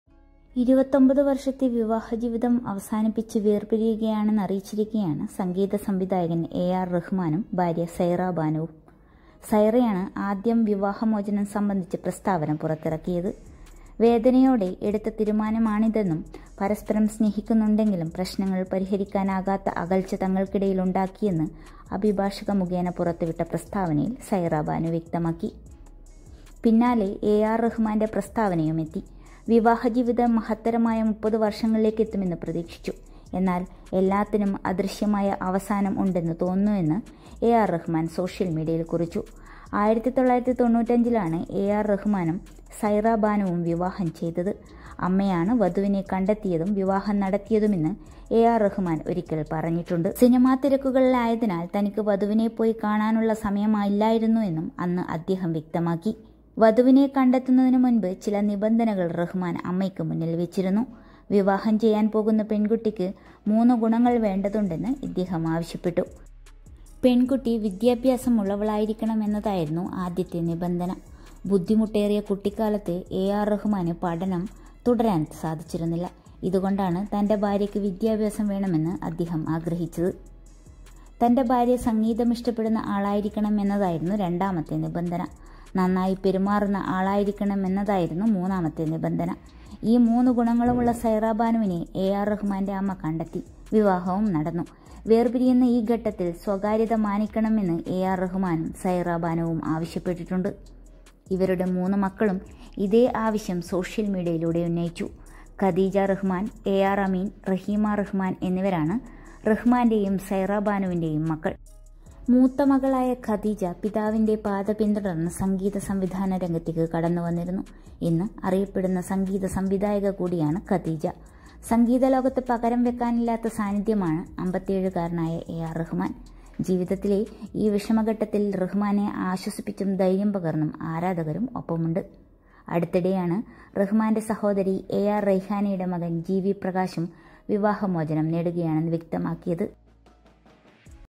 ആകെ തകർന്ന സാഹചര്യമെന്ന് എ.ആര്‍. റഹ്‌മാന്‍; കൂപ്പുകയ്യുമായി മകള്‍ |ARRahmandivorce | sairabanu
Arrahman sairabanu seperation💔
"30 വർഷം ഒരുമിച്ച് തികയ്ക്കാനാവാഞ്ഞതിൽ ദുഃഖം"
1995 ലാണ് എആർ റഹ്മാനും സൈറ ബാനുവുവും വിവാഹിതരായത്. എല്ലാത്തിനും അദൃശ്യമായ അവസാനം ഉണ്ടെന്ന് തോന്നുന്നെന്നാണ് എആർ റഹ്മാൻ സോഷ്യൽ മീഡിയയിൽ കുറിച്ചത്.